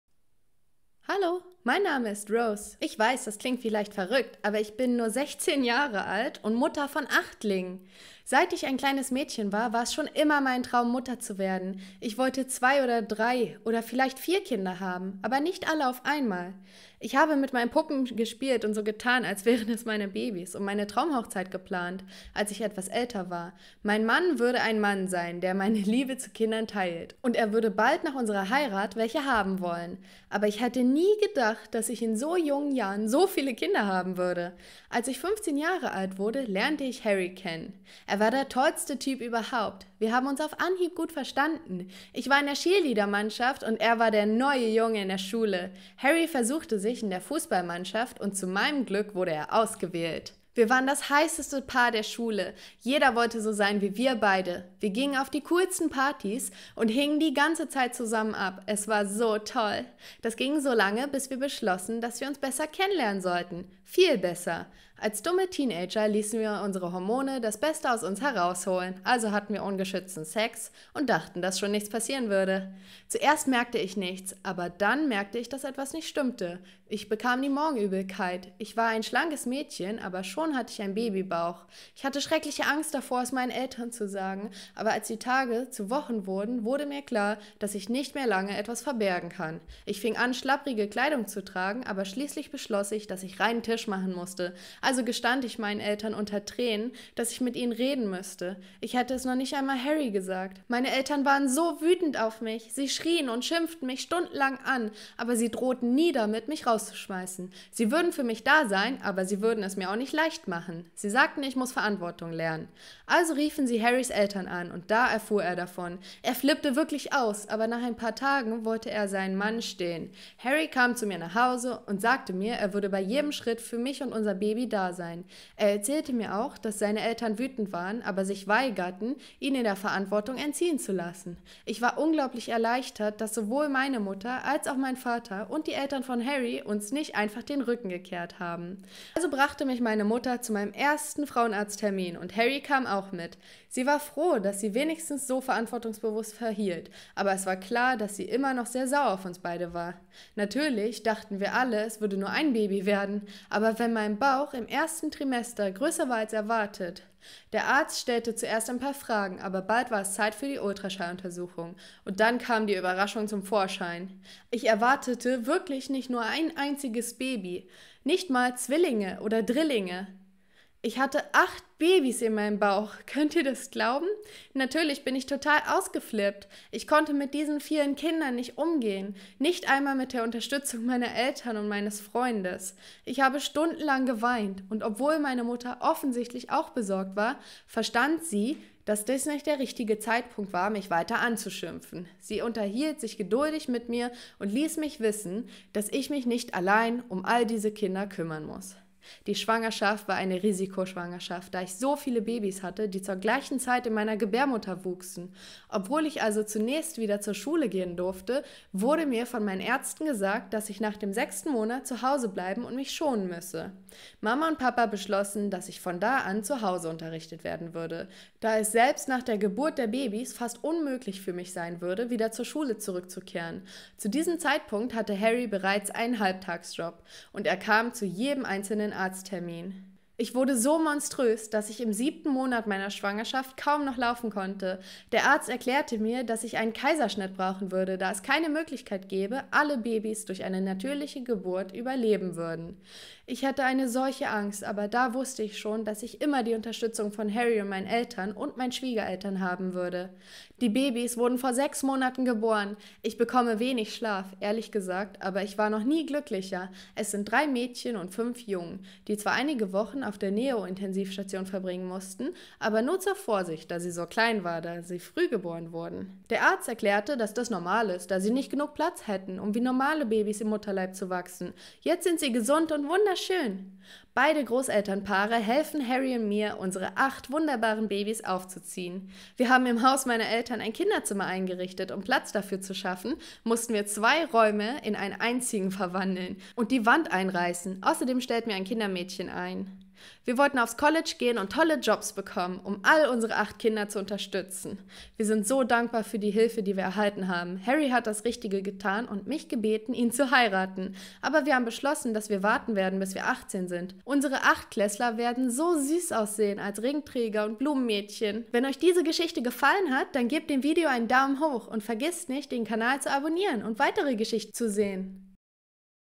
Ich bin mit 16 Mutter von Achtlingen
Ich bin mit 16 Mutter
von Achtlingen
Hallo! Mein Name ist Rose. Ich weiß, das klingt vielleicht verrückt, aber ich bin mit nur 16
Jahren Mutter von Achtlingen!
Seit ich ein kleines Mädchen war, war es schon immer mein Traum, Mutter zu werden. Ich
wollte zwei oder drei... oder vielleicht vier Kinder haben, aber nicht alle auf einmal!
Ich habe mit meinen Puppen gespielt und so getan, als wären es meine Babys, und meine
Traumhochzeit geplant, als ich etwas älter war. Mein Mann würde ein Mann sein, der meine
Liebe zu Kindern teilt, und er würde bald nach unserer Heirat welche haben wollen!
Aber ich hätte nie gedacht, dass ich in so jungen Jahren so viele Kinder haben würde!
Als ich 15 Jahre alt wurde, lernte ich Harry kennen. Er war der tollste Typ überhaupt! Wir haben
uns auf Anhieb gut verstanden. Ich war in der Cheerleader-Mannschaft, und er war der neue
Junge in der Schule. Harry versuchte sich in der Fussballmannschaft, und zu meinem Glück
wurde er ausgewählt!
Wir waren das heißeste Paar der Schule, jeder wollte so sein wie wir beide. Wir gingen auf die
coolsten Partys und hingen die ganze Zeit zusammen ab, es war so toll.
Das ging so lange, bis wir beschlossen, dass wir uns besser kennenlernen wollten. Viel
besser... Als dumme Teenager ließen wir unsere Hormone das Beste aus uns herausholen.
Also hatten wir ungeschützten Sex und dachten, dass nichts passieren würde.
Zuerst merkte ich nicht, dass etwas nicht stimmte. Dann begann die Morgenübelkeit. Ich war
immer ein schlankes Mädchen, und so machte sich mein Babybauch schon sehr bald
bemerkbar! Ich hatte schreckliche Angst davor, es meinen Eltern zu sagen, aber als die Tage
zu Wochen wurden, wurde mir klar, dass ich es nicht mehr lange verbergen konnte.
Ich fing an, schlabberige Kleidung zu tragen, aber schließlich beschloss ich, dass ich reinen
Tisch machen musste. Also gestand ich meinen Eltern unter Tränen, dass ich mit ihnen reden
müsse. Ich hatte es noch nicht einmal Harry gesagt!
Meine Eltern waren so wütend auf mich! Sie schrien und schimpften mich stundenlang, aber sie
drohten nie damit, mich rauszuschmeißen. Sie würden für mich da sein, aber sie würden es mir
auch nicht leicht machen. Sie sagten, ich müsse etwas Verantwortung lernen!
Also riefen sie Harrys Eltern an, und da erfuhr er davon. Er flippte wirklich aus, aber nach ein
paar Tagen wollte er seinen Mann stehen. Harry kam zu mir nach Hause, und er sagte mir, er

würde bei jedem Schritt für mich und unser Baby da sein.
Er erzählte mir auch, dass seine Eltern wütend waren, aber sich weigerten, ihn sich der
Verantwortung entziehen zu lassen.
Ich war unglaublich erleichtert, dass sowohl meine Mutter als auch mein Vater, und die Eltern
von Harry, uns nicht einfach den Rücken gekehrt haben.
Also brachte mich meine Mutter zu meinem ersten pränatalen Termin, und Harry kam auch mit.
Sie war froh, dass er sich wenigstens so verantwortungsbewusst verhielt, aber es war klar,
dass sie immer noch sehr sauer auf uns beide war.
Natürlich dachten wir alle, es würde nur ein Baby werden, auch wenn mein Bauch im ersten
Trimester größer war als erwartet. Der Arzt stellte zuerst ein paar Fragen, aber bald war es Zeit
für die Ultraschalluntersuchung, und dann kam die große Überraschung zum Vorschein!
Ich erwartete nicht nur ein Baby. Nicht einmal Zwillinge oder Drillinge! Ich hatte acht Babys in
meinem Bauch! Könnt ihr das glauben?!
Natürlich bin ich total ausgeflippt! Ich konnte mit diesen vielen Kindern nicht umgehen, nicht
einmal mit der Unterstützung meiner Eltern und meines Freundes!
Ich habe stundenlang geweint, und obwohl meine Mutter offensichtlich auch besorgt war,
verstand sie, dass es nicht der richtige Zeitpunkt war, mich weiter zu schimpfen. Sie unterhielt
sich geduldig mit mir und ließ mich wissen, dass ich mich nicht allein um all diese Kinder
kümmern müsse.
Die Schwangerschaft war eine Risikoschwangerschaft, da ich so viele Babys hatte, die zur
gleichen Zeit in meiner Gebärmutter wuchsen. Obwohl ich also zunächst wieder zur Schule
gehen durfte, wurde mir von meinen Ärzten gesagt, dass ich nach dem 6. Monat zu Hause
bleiben und mich schonen müsse.
Mama und Papa beschlossen, dass ich von da an zu Hause unterrichtet werden würde, da es
selbst nach der Geburt der Babys fast unmöglich für mich sein würde, wieder zur Schule
zurückzukehren. Zu diesem Zeitpunkt hatte Harry bereits einen Halbtagsjob, und er kam zu
jedem einzelnen Arzttermin.
Ich wurde so monströs, dass ich im siebten Monat meiner Schwangerschaft kaum noch laufen
konnte! Der Arzt erklärte mir, dass ich einen Kaiserschnitt brauchen würde, da es keine
Möglichkeit gäbe, dass alle Babys eine natürliche Geburt überleben würden.